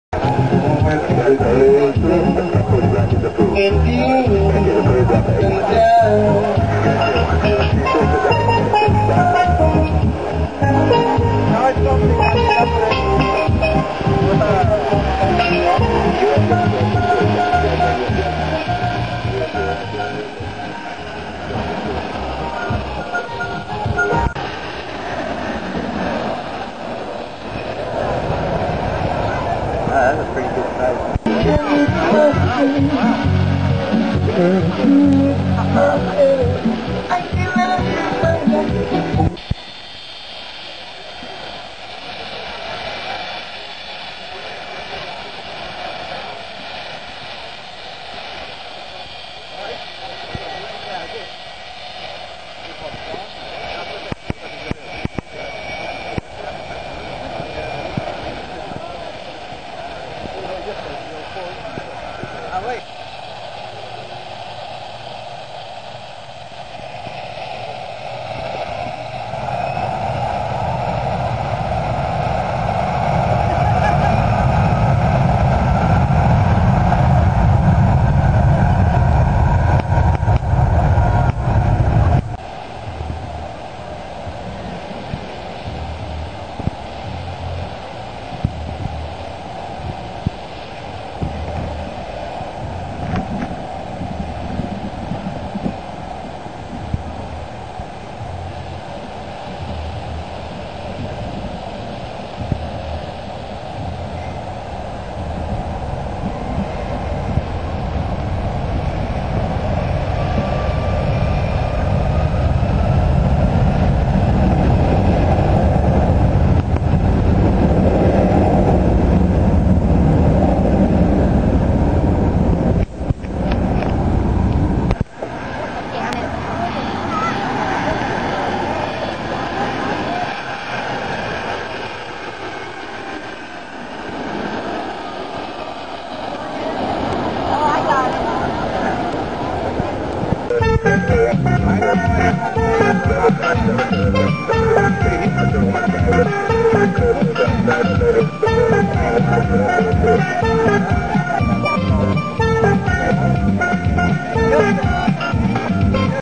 Philipsburg Airport, St. Maarten
the famous airport in St. Maarten. Planes land right over sunset beach. Beachgoers are sandblasted by jet wash from departing aircraft.